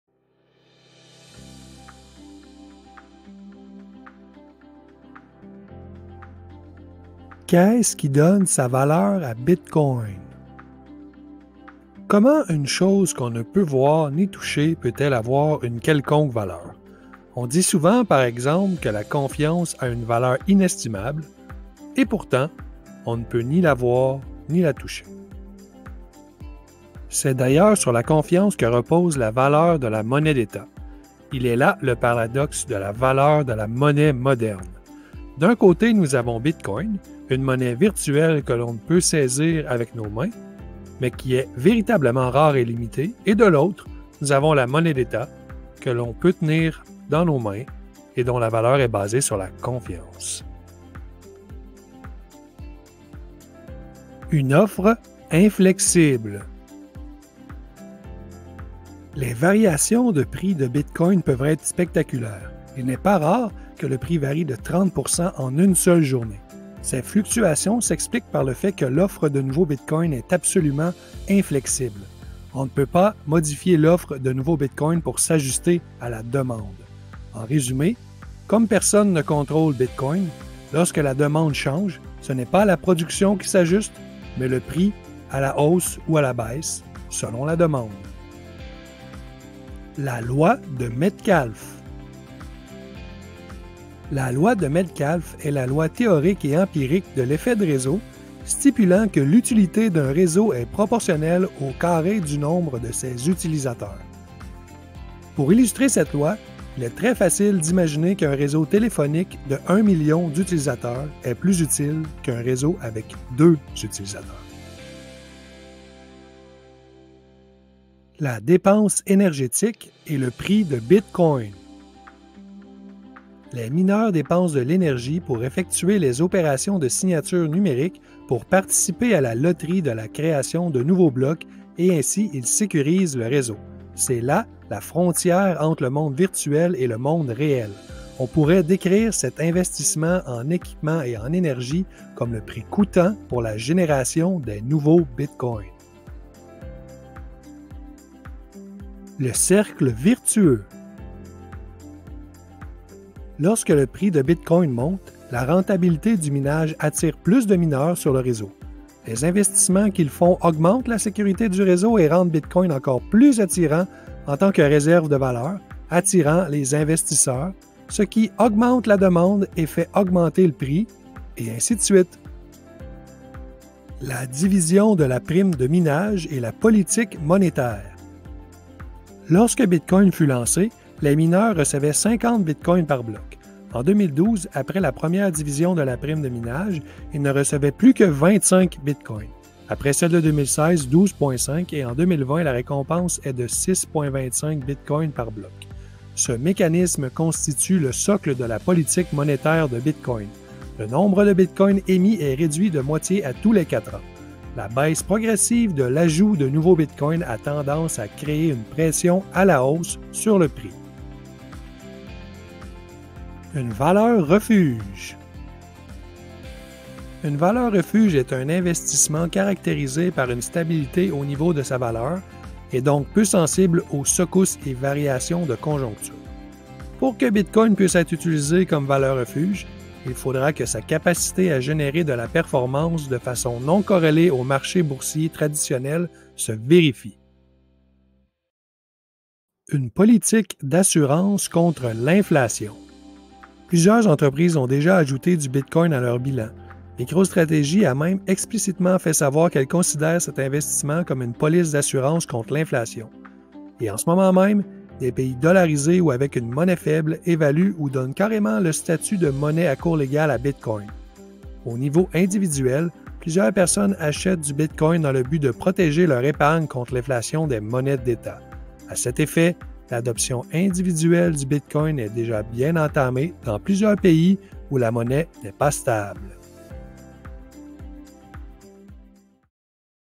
Qu'est ce qui donne sa valeur à Bitcoin ? - Doodling Bitcoin #4
Comment une chose qu’on ne peut ni voir ni toucher peut-elle avoir une quelconque valeur ? Découvrons ensemble ce qui donne sa valeur à Bitcoin.
Le contenu de cette vidéo est inspiré du livre Tout sur Bitcoin de David St-Onge